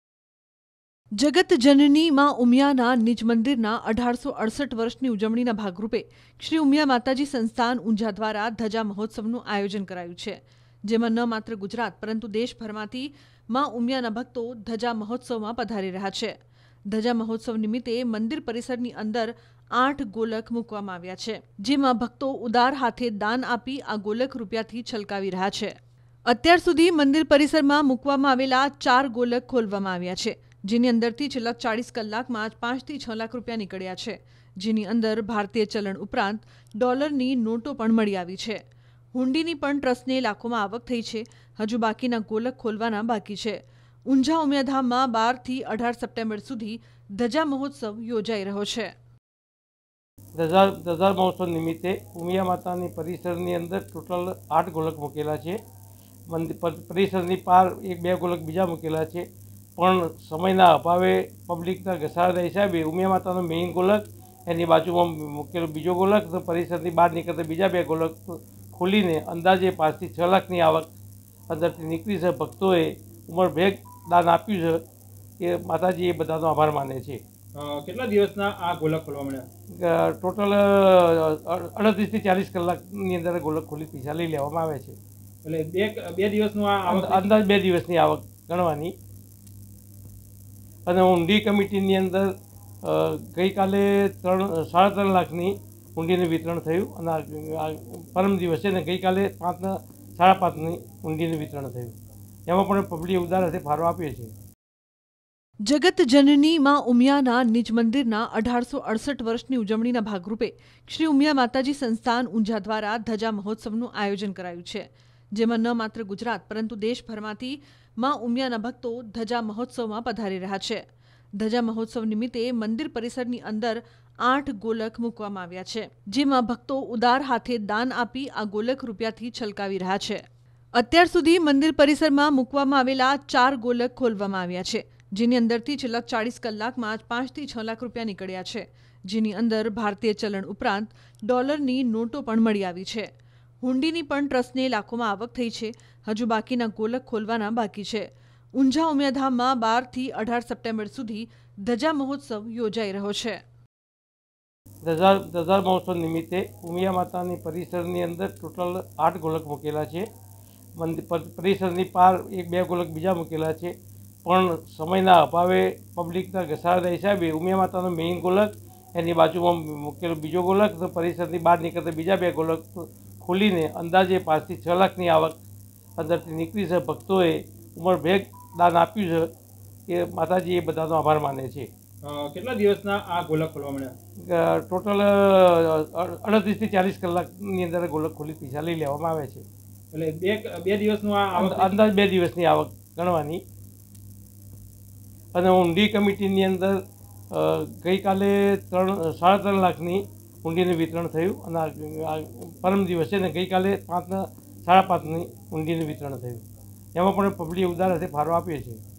Mehsana News | ઉમિયાધામ ધજા મહોત્સવ ! 4 ગોલખમાંથી નીકળ્યા 5થી 6 લાખ | Unjha | Umiya Dham | N18V
As part of the celebration of 1868 years of Umiya's own temple in Jagat Janani, Dhaja Mohotsav has been organized by Shri Umiya Mataji Sansthan Unjha.

જગત જનની માં ઉમિયાના નિજ મંદિરના 1868 વર્ષની ઉજવણીના ભાગરૂપે શ્રી ઉમિયા માતાજી સંસ્થાન ઊંઝા દ્વારા ધજા મહોત્સવનું આયોજન કરાયું છે. n18oc_mehsana